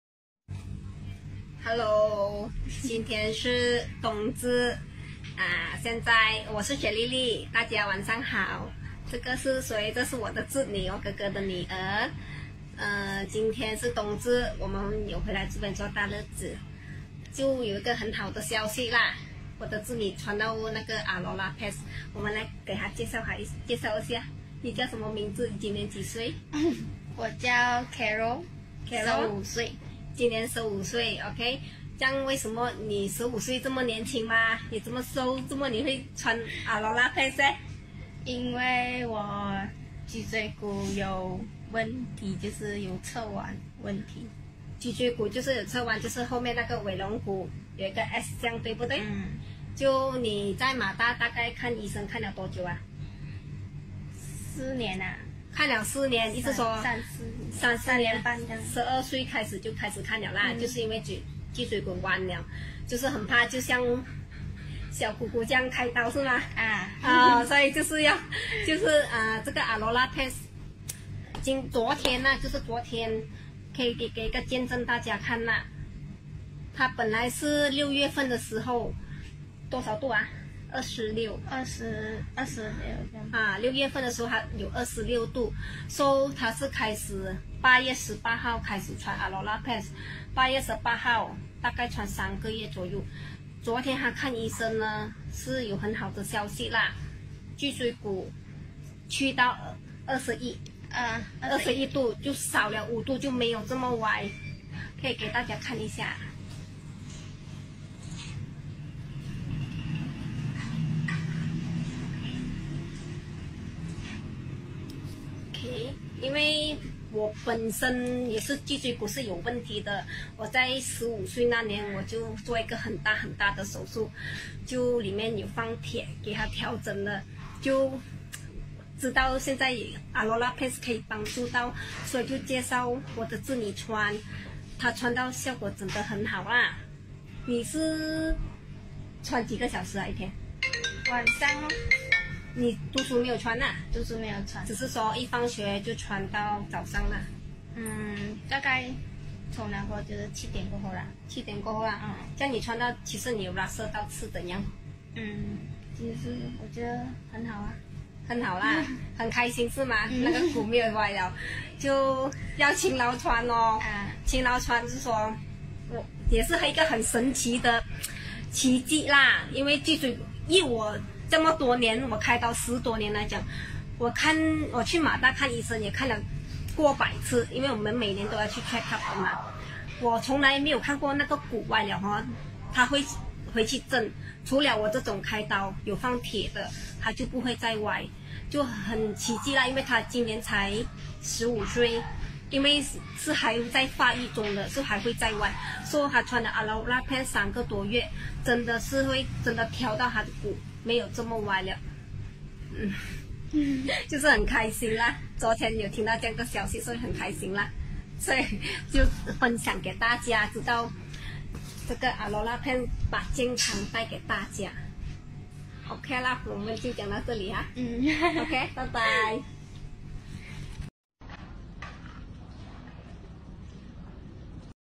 Aulora Kodenshi Pants 改善 脊椎骨折歪问题
来听听，来自PD的小妹妹Carol Lee的分享💖

Carol 在12岁时就发现脊椎骨折歪问题，今年2018的六月份时看医生X-Ray Report👉 是26度👈都很担心还会继续歪，经过婷姑姑的介绍之下，就买一件来穿。

哇哇哇！惊喜来啦！
她从18/8/18年开始穿上AULORA KODENSHI PANTS 很开心的是21/12/18她又到回马大医院check up 。。

X-Ray Report 出来的是
✨ 21度 ✨比之前好了💖5💖度哦！她再看清楚真的是💞5💞度哦！真的真的非常开心😃一件裤既然可以有意想不到的效果👍👍👍

❤Aulora Kodenshi Pants不止裤那么简单

如果里面内容帮到你，请分享给你的家人与朋友!

想了解，欢迎联系我 ( Amanda Koh) 询问更多详情。

🌷Amanda Koh 🌷